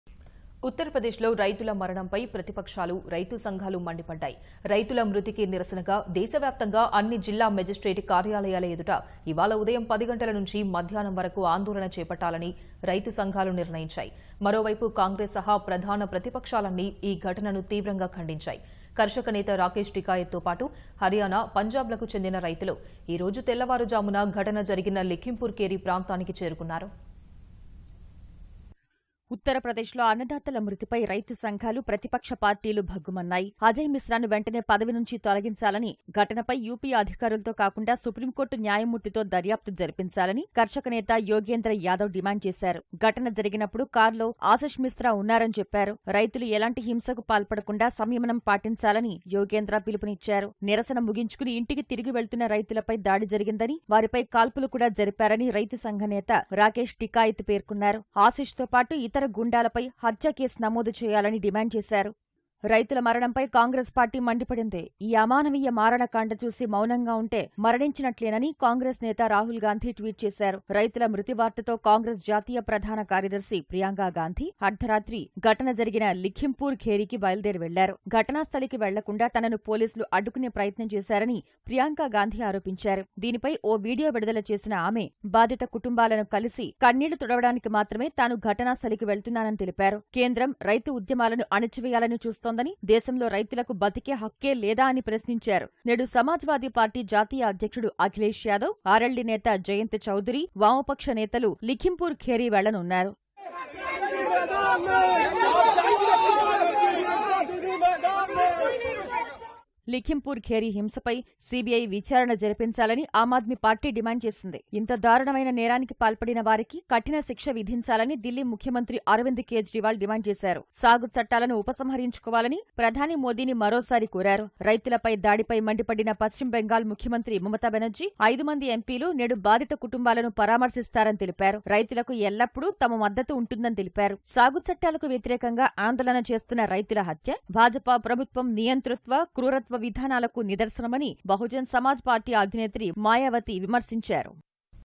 Mamata Banerjee, Congress Leaders Slam BJP Over UP Violence | ప్రతిపక్షాలు, రైతు సంఘాలు ఆందోళన
ఉత్తరప్రదేశ్ లో రైతుల మరణంపై ప్రతిపక్షాలు, రైతు సంఘాలు మండిపడ్డాయి. రైతుల మృతికి నిరసనగా...దేశవ్యాప్తంగా అన్ని జిల్లా మేజిస్ట్రేట్ కార్యాలయాల ఎదుట ఇవాళ ఉదయం పది గంటల నుంచి మధ్యాహ్నం వరకు ఆందోళన చేపట్టాలని...రైతు సంఘాలు నిర్ణయించాయి. మరోవైపు కాంగ్రెస్ సహా...ప్రధాన ప్రతిపక్షాలన్నీ ఈ ఘటనను తీవ్రంగా ఖండించాయి. కర్షక నేత రాకేశ్ టికాయిత్ తోపాటు...హరియాణా, పంజాబ్ లకు చెందిన రైతులు...ఈరోజు తెల్లవారుజామున...ఘటన జరిగిన లఖింపుర్ ఖేరి ప్రాంతానికి చేరుకున్నారు